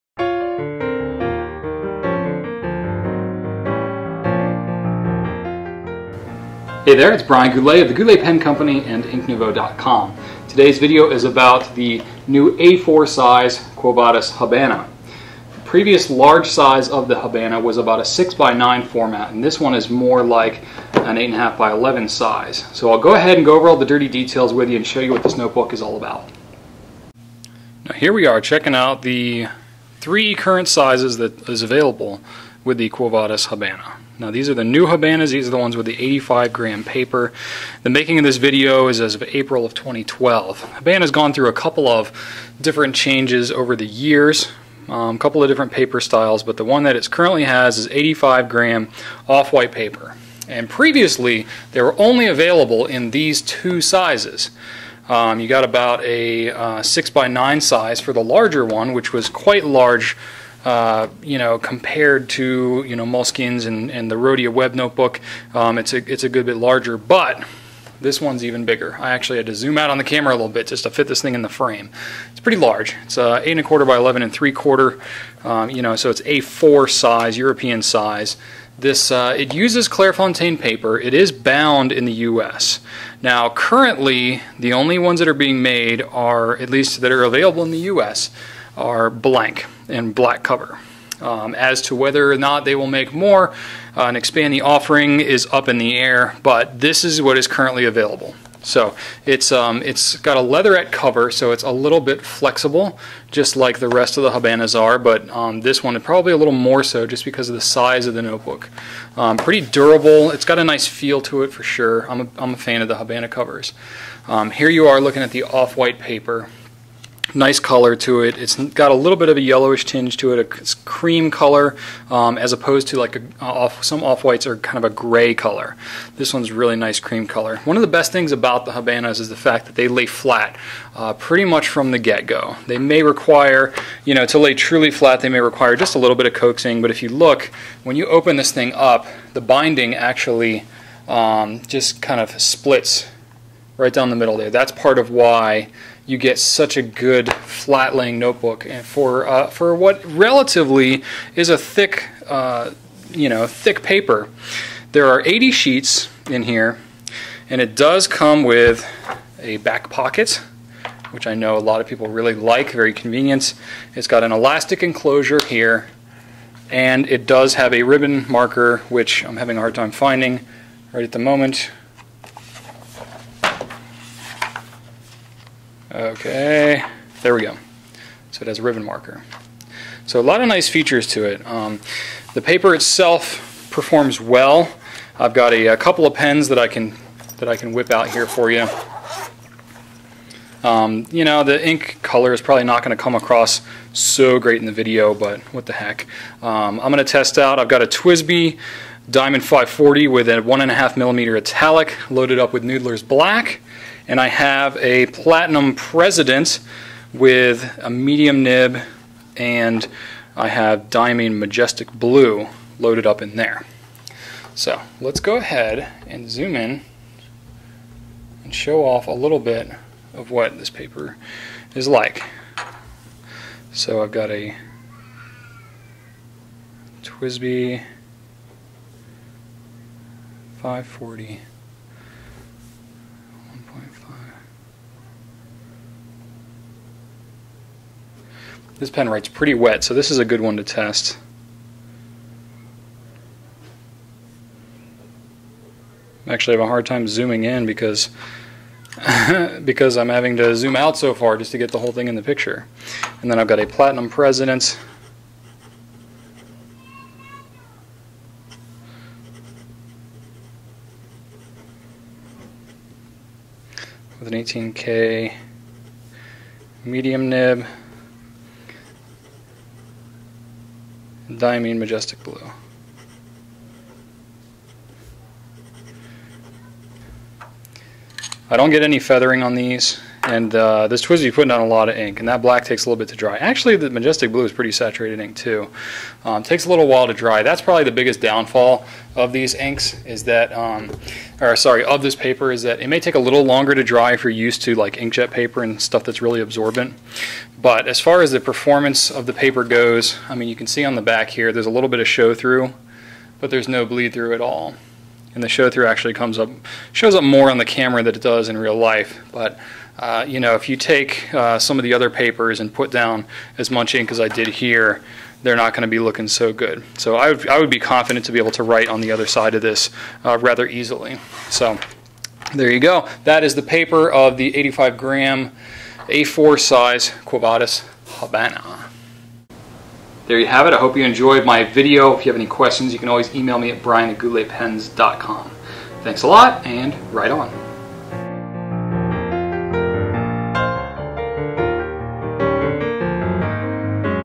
Quo Vadis Habana A4 Blank
Quo Vadis now has an A4 blank bound Habana journal available in the US. 85g paper, very fountain pen friendly, and lays very flat. Great A4 journal, if this is what you were waiting for.